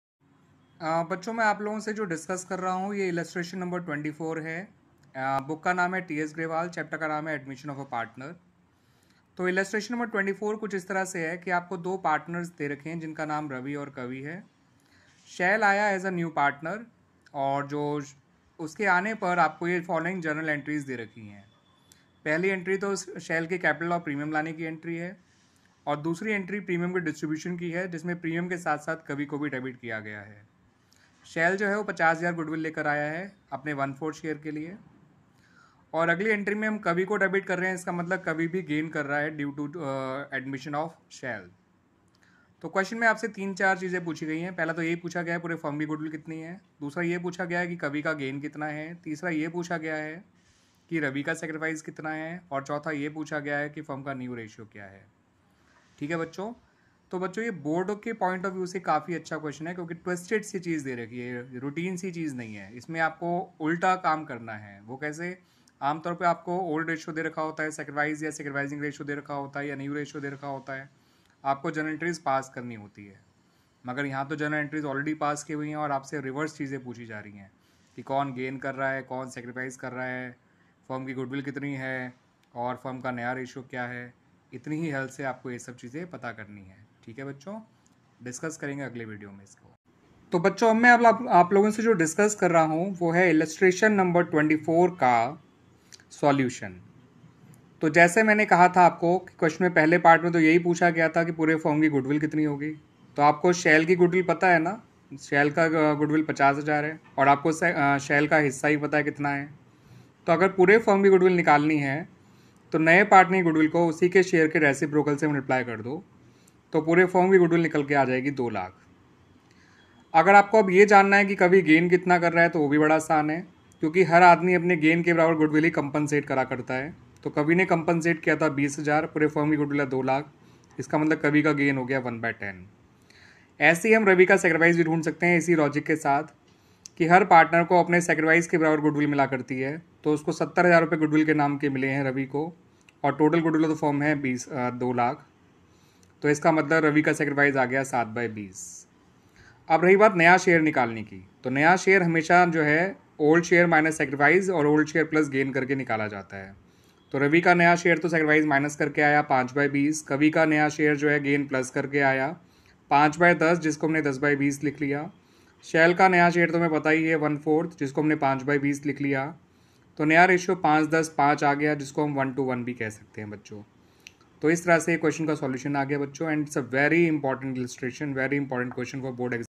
Illustration 24 | Admission of a partner | class xii | Ts grewal | Edition 2022
in this video i have explained the solution of illustration 24 of chapter admission of a partner Ts grewal Edition 2022

Ravi and Kavi were partners in a firm sharing profits in the ratio of 3:2 . They admit Sahil into partnership for 1/4 share in profits. The partners passed the following Journal entries at the time of admission of Sahil :
Bank.  A/c dr. 300000
to Sahil's capital A/c 250000
to premium for goodwill A/c 50000

Premium for Goodwill A/c dr. 50000
Kavi's capital a/c dr. 20000
to Ravi's capital account

Calculate i Firm's Goodwill , ji Sacrifice /Gain to Ravi and Kavi on Sahil's admission ; and iii New Profit-sharing Ratio